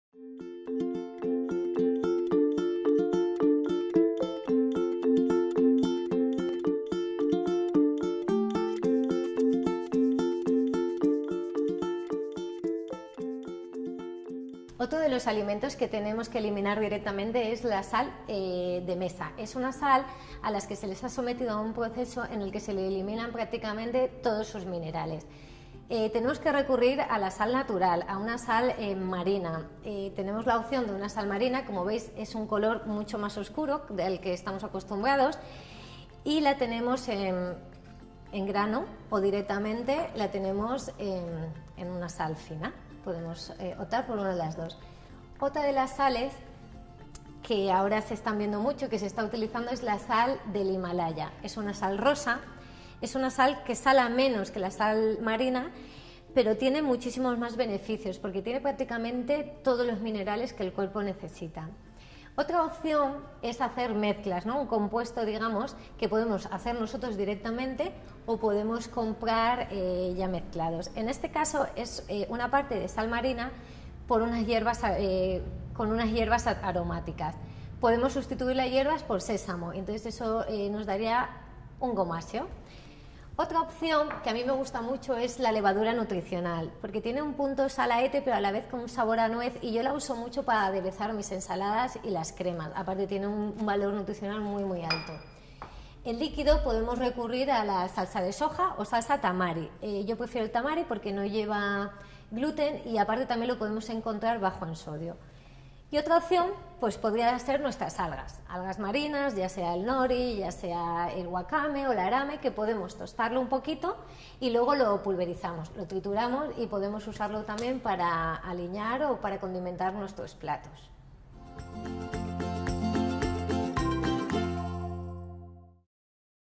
ApuntoConSKEYNDOR | Lou Mateos | Trucos para hacer recetas sabrosas con menos sal
Hábitos saludables con Lou Mateos. Trucos para hacer recetas sabrosas con menos sal.